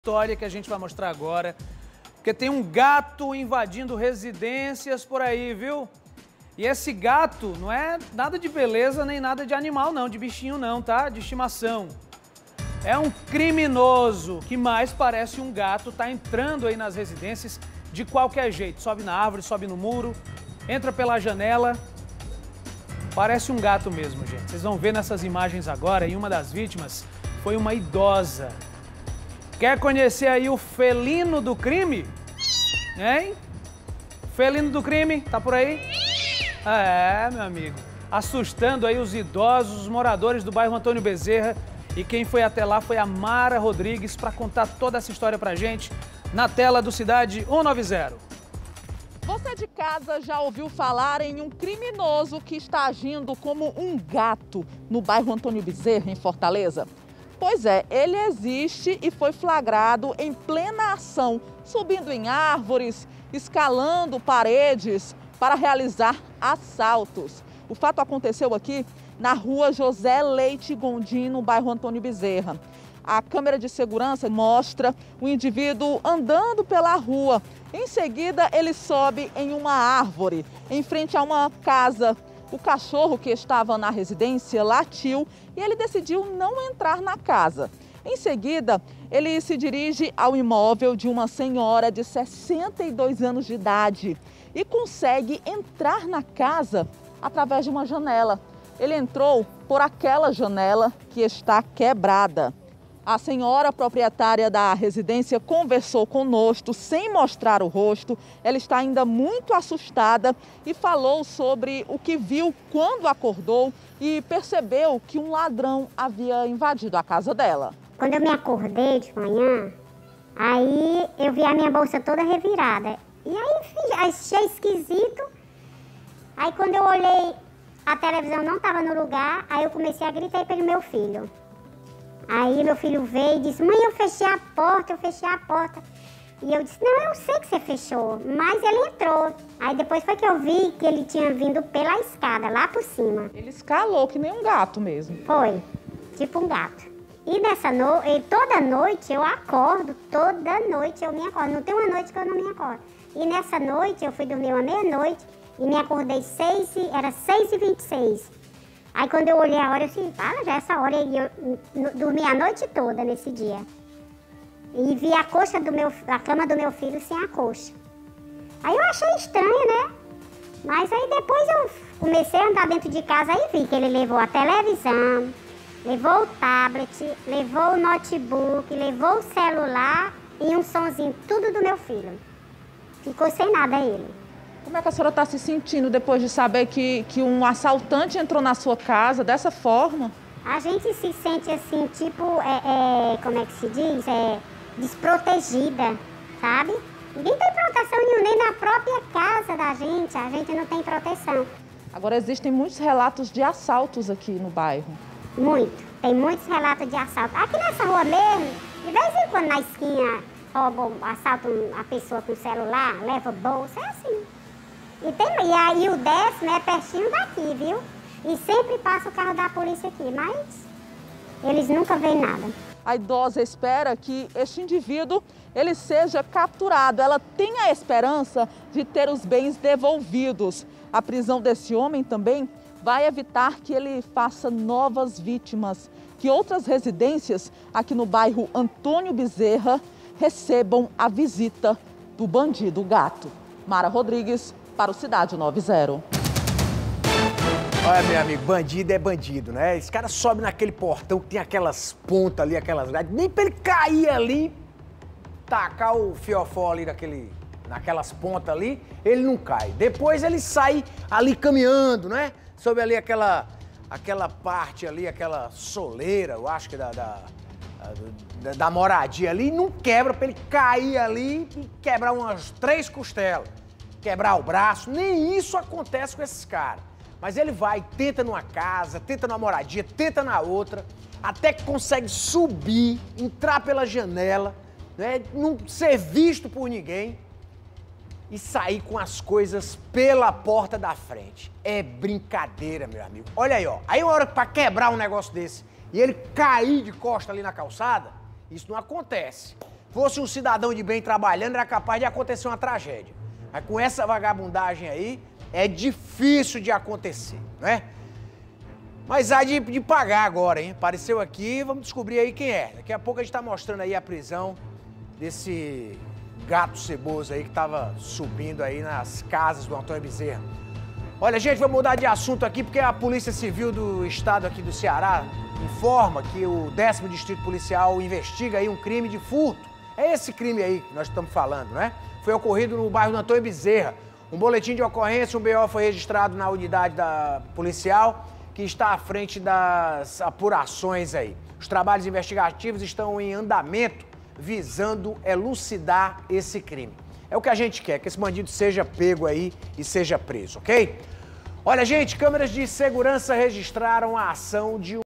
Bandido age como gato e pratica furto em residência de idosa
Você que mora no bairro Antônio Bezerra e adjacências, presta atenção nessa história que vamos mostrar agora. Tem gato invadindo residências pra praticar furtos. Câmeras de segurança registraram a ação. Uma das vitimas foi uma idosa. Quer conhecer o 'felino do crime'? Mara Rodrigues mostra pra gente.